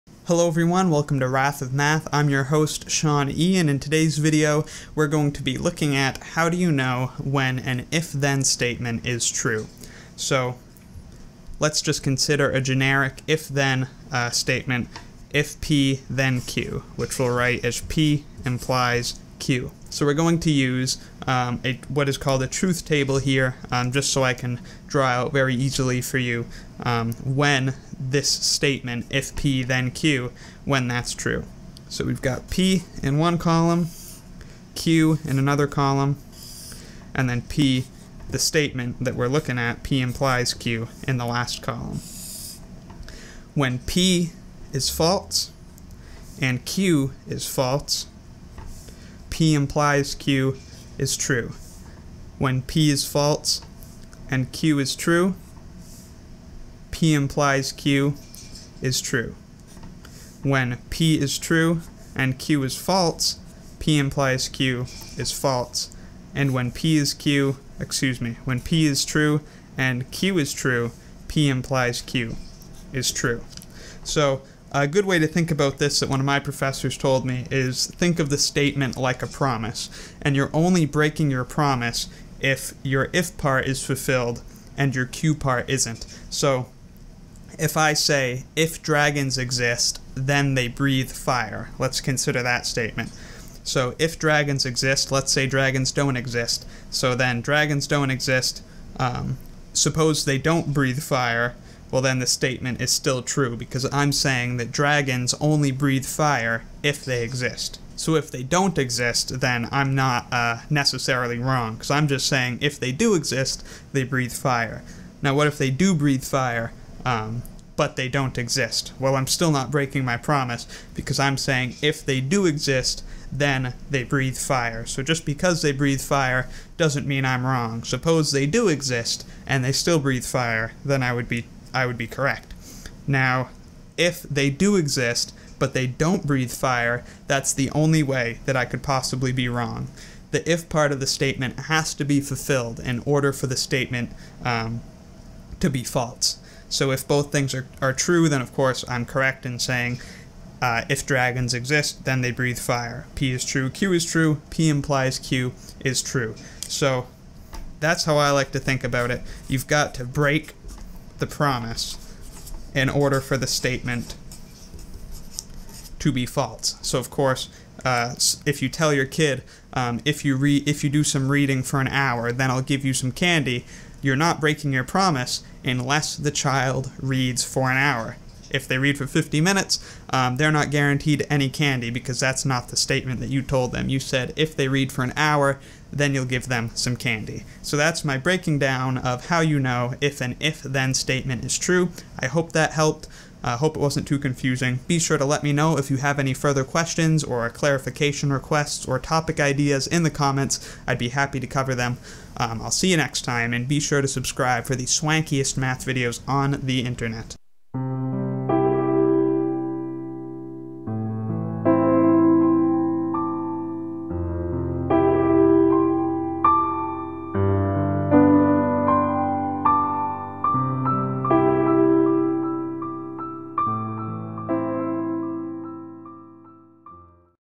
When is an If-Then Statement True? (Conditional Statements)
What is an if then statement? Also known as a conditional statement, if the if condition is fulfilled then the then part of the statement is supposed to occur. If it does not occur, then the statement is false. When is a conditional statement true? Well, we go over all of that in this brief lesson using a truth table to show when an "if this then that" statement is true. I hope you find this lesson helpful, and let me know if you would like to see more conditional statement examples.